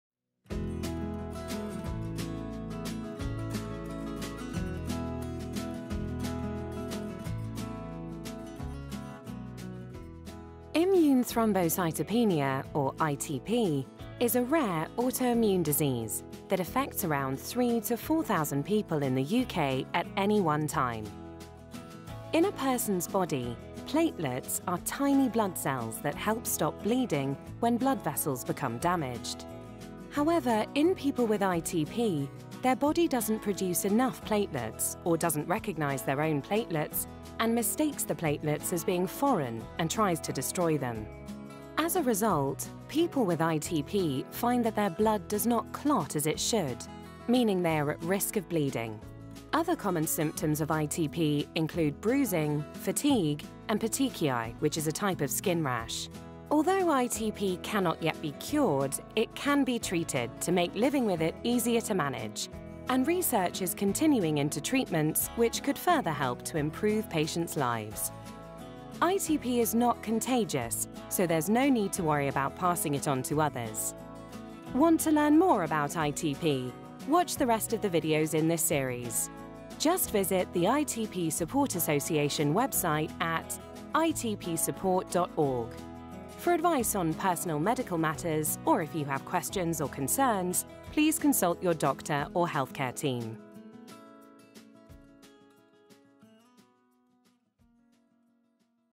What is ITP
mmune thrombocytopenia (ITP) is a rare autoimmune disease that stops your blood from clotting properly which can result in excessive bleeding and bruising